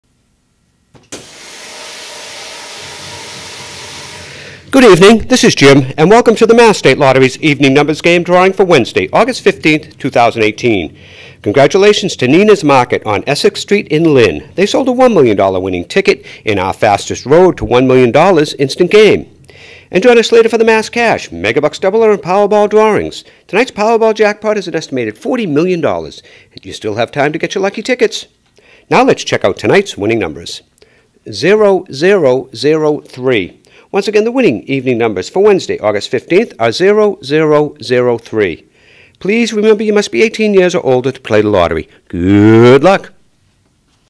Evening Numbers Game Drawing: Wednesday, August 15, 2018
The official winning numbers in the Mass. State Lottery's Evening Numbers Game drawing for Wednesday, August 15, 2018 are: 0-0-0-3.